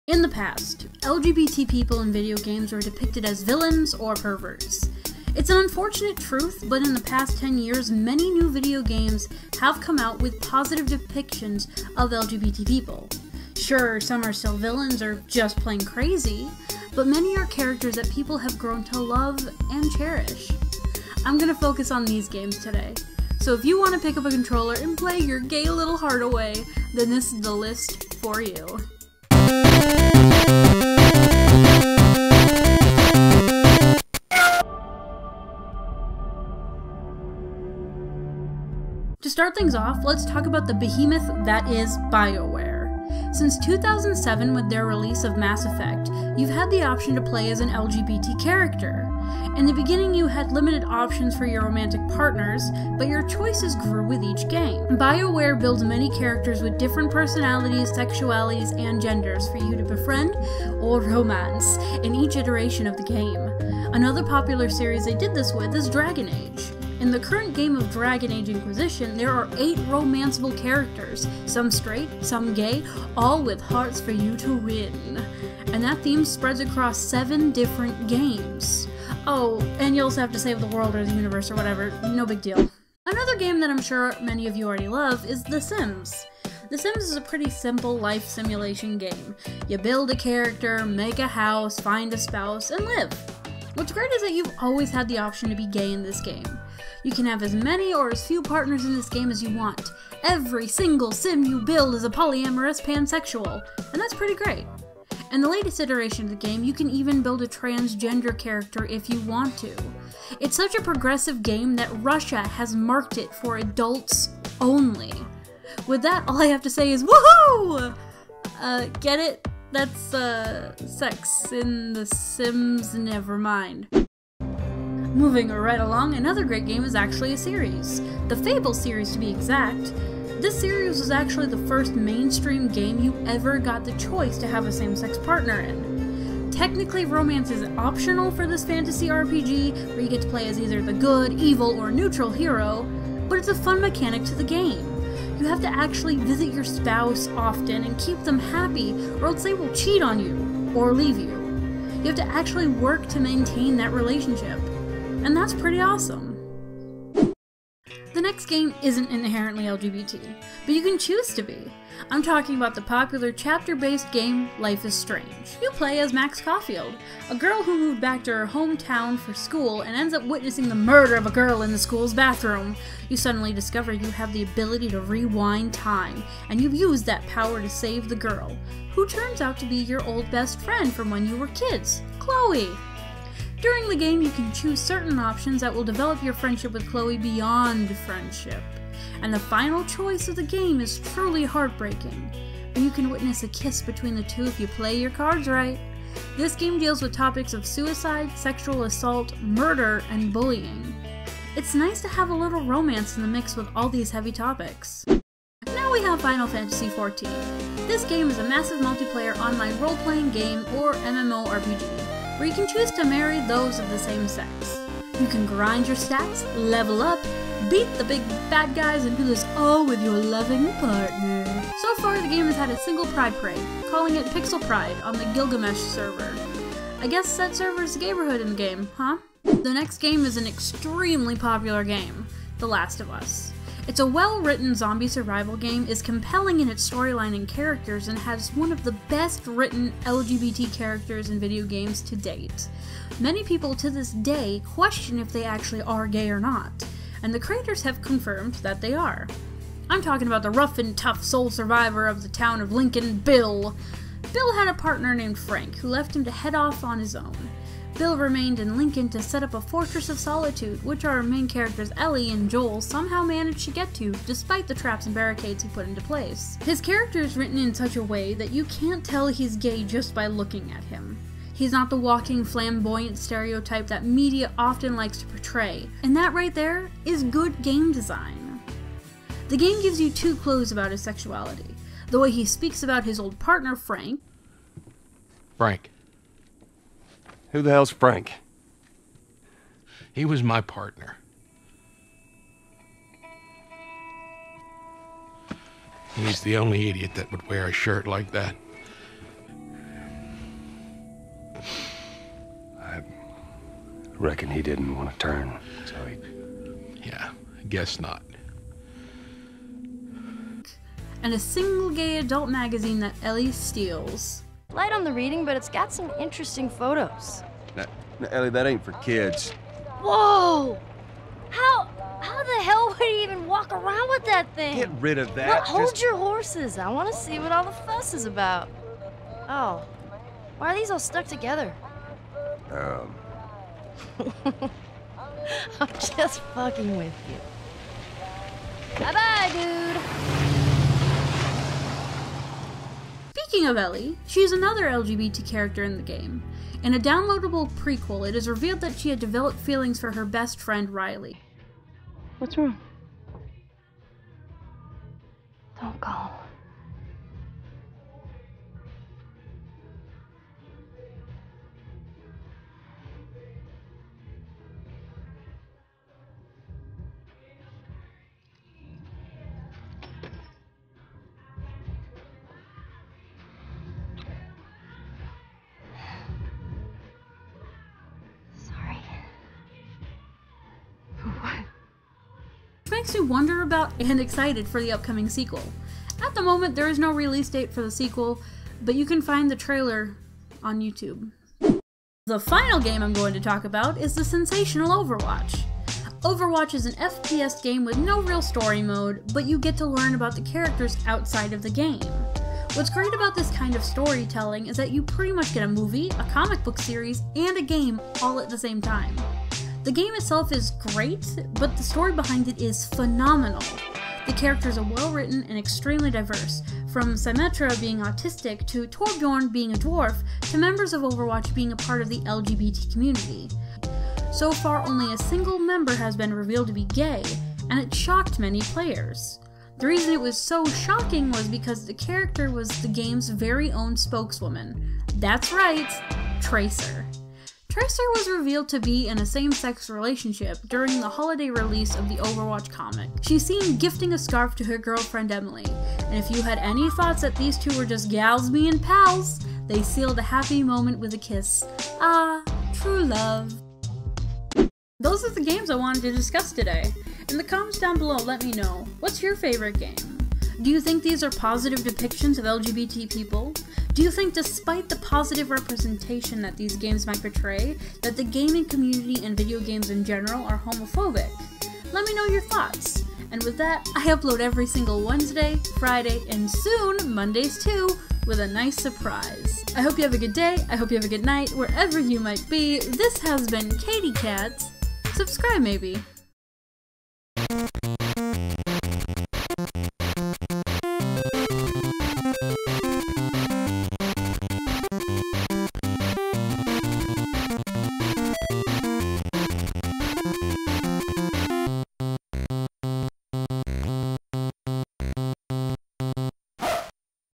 THE BEST LGBT VIDEO GAMES
Here is a list of some of the best LGBT games!

Whoops, it's Thursday. Would you buy the excuse that I was building a table and that's why the video is late? Haha